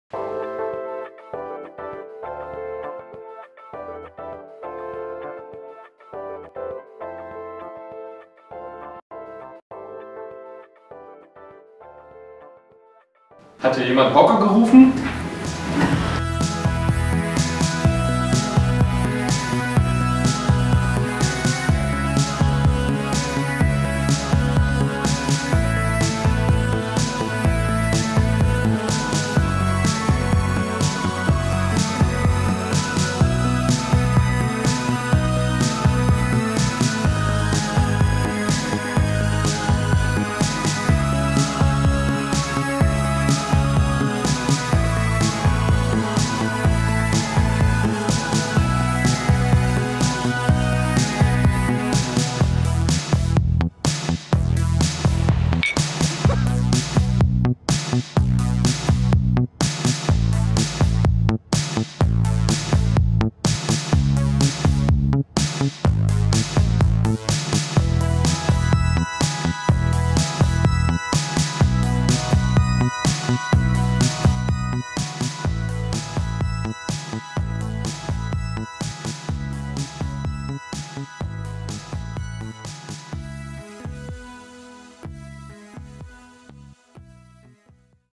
SWE Hocker für Erfurt
Mit unseren SWE Hockern entspannt sitzen - nicht nur beim Sinfoniekonzert am 17. Juni 2016.